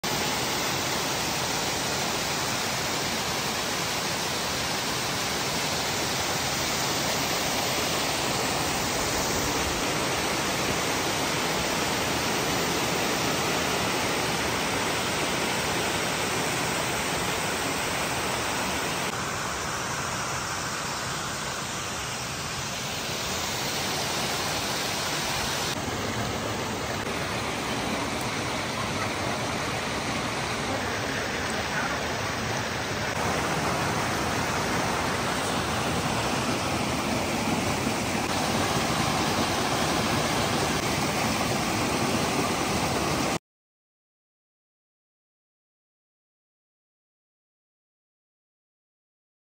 Experience the Rhythm of Water Flow at Caozo WaterFall in Spain | Wonder of Nature
This waterfall is within the Garganta de Bonal between Valdetillas and Piornal.